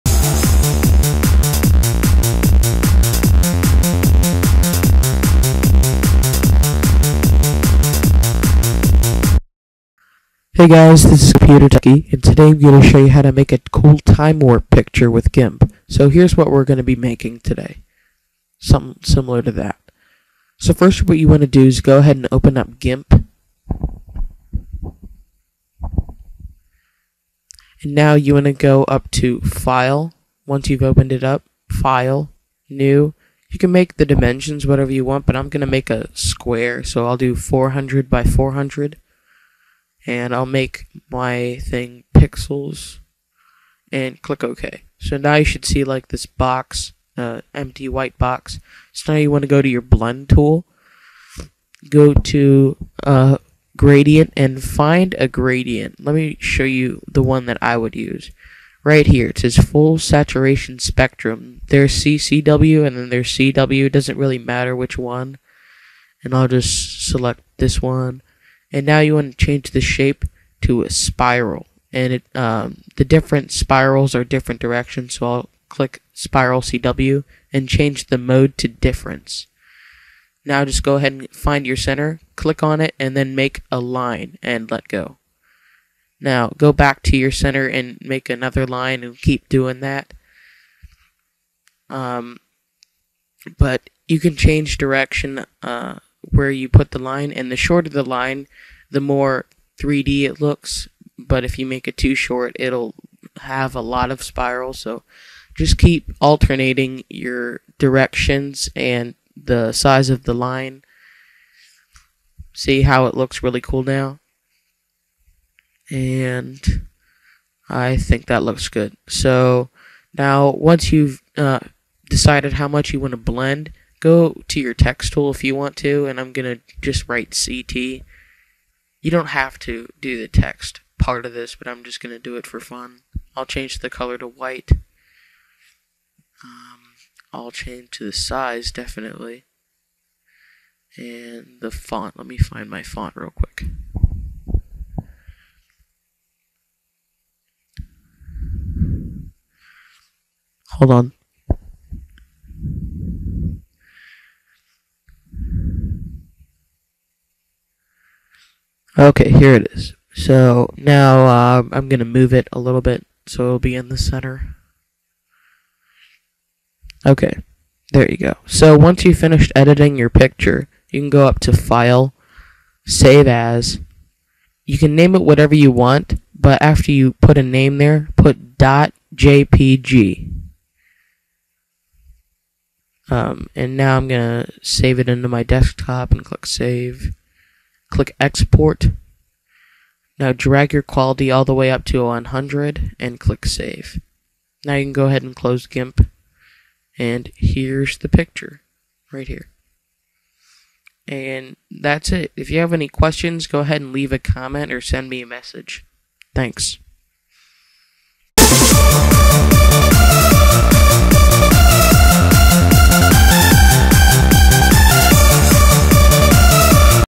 How to Make a cool Time-warp image using GIMP
This video explains how to make a cool color blend that looks similar to  a "Time Warp" using GIMP.  It's pretty cool.

Intro/Outro Music by NAL1200